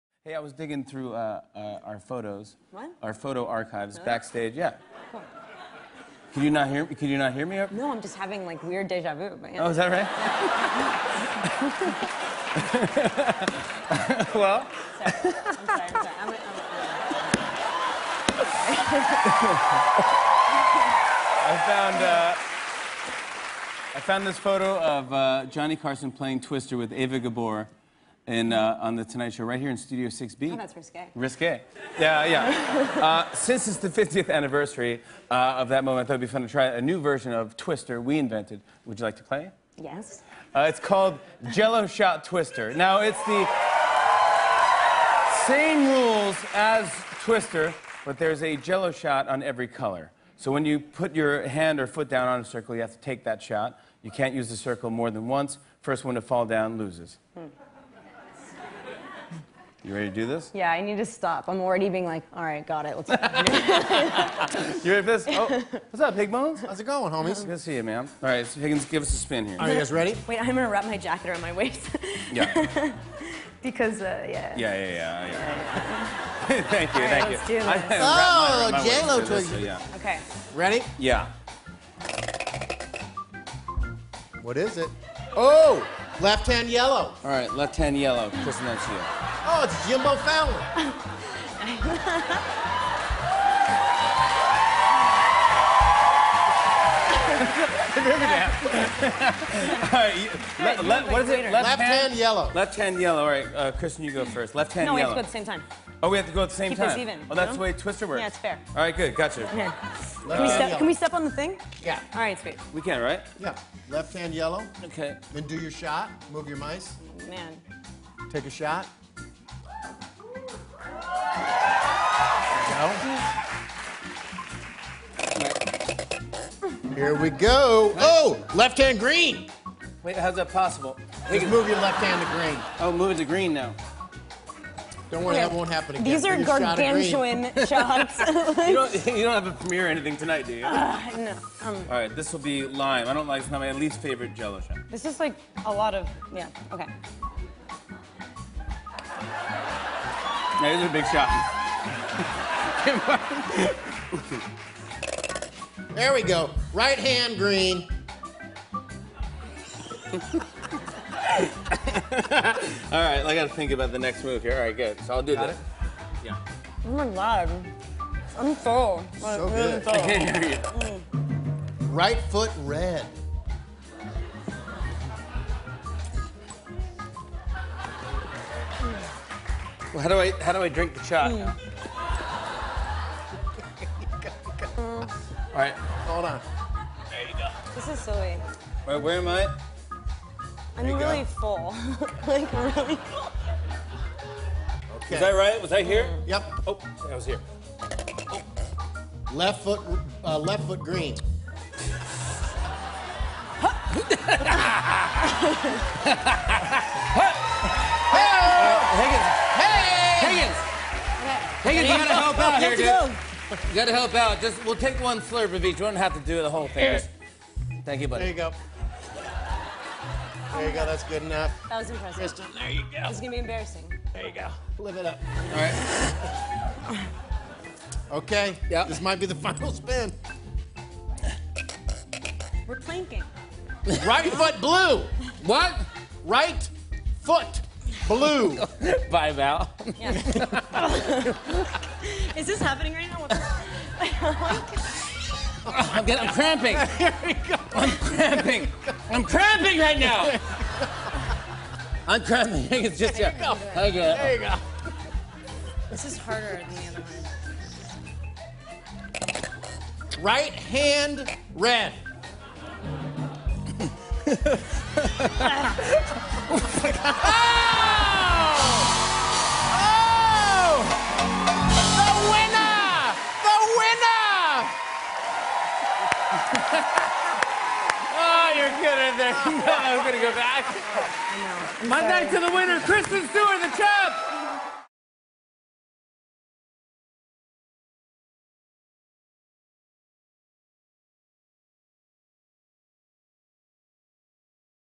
Jell-O Shot Twister with Kristen Stewart
Jimmy and Kristen Stewart face off in a game of Twister where they have to take a Jell-O shot every time they place a body part on a colored spot.